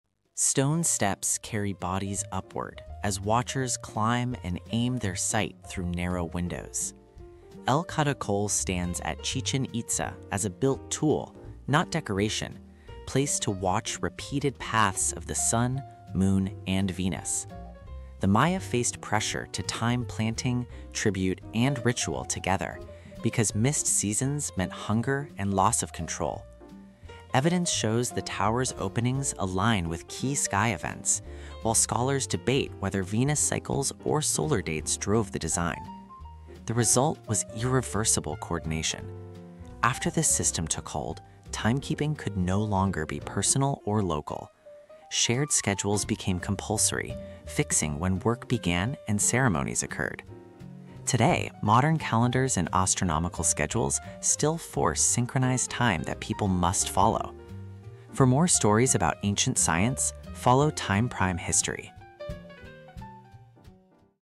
El Caracol Observatory
A Maya observatory that aligned buildings to the sky, enforcing shared calendars that organized labor, ritual, and authority.
For more stories about ANCIENT SCIENCE, follow Time Prime History 📌